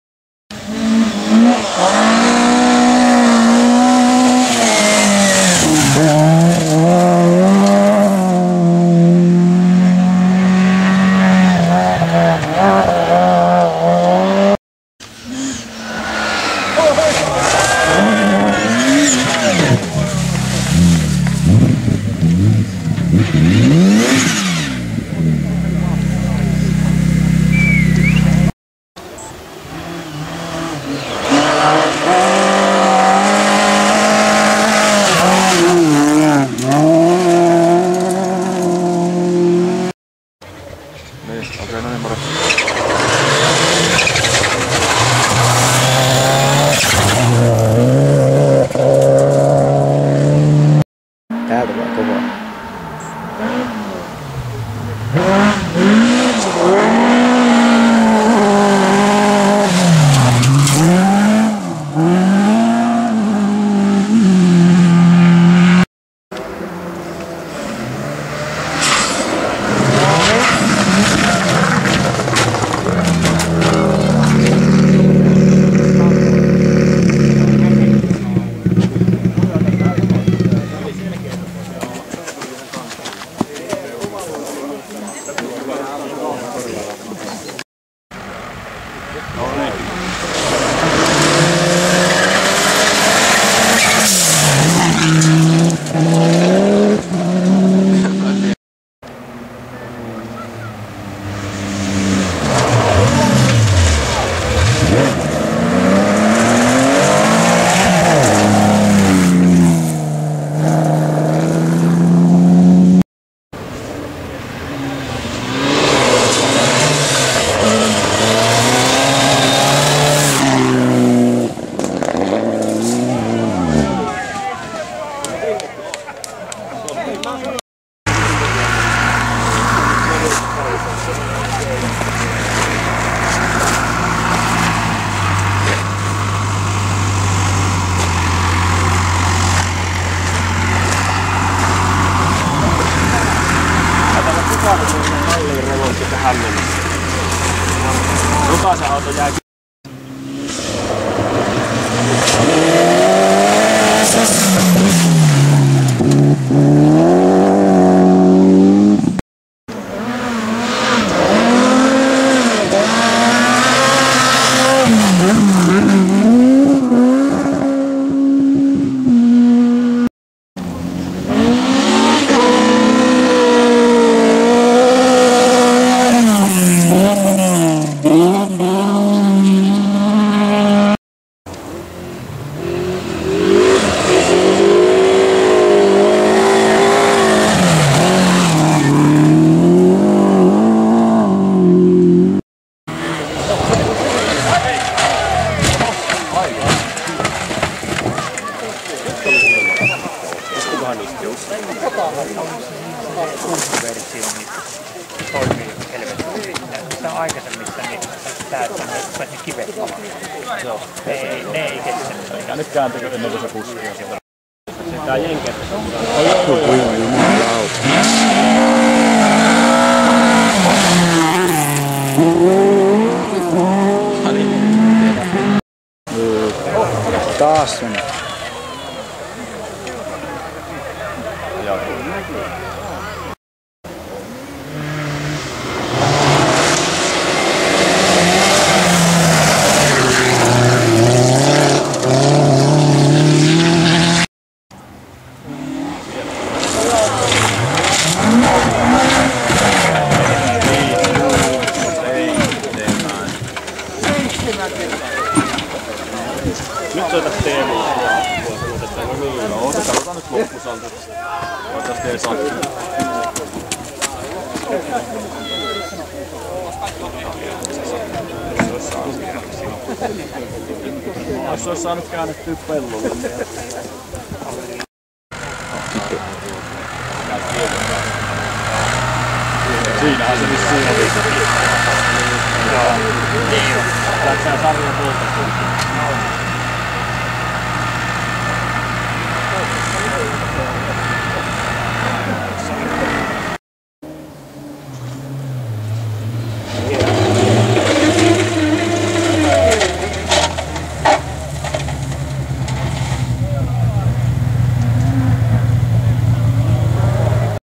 Ovisepät rallisprint 17.9.2011
3 kaatoa ja ulosajoja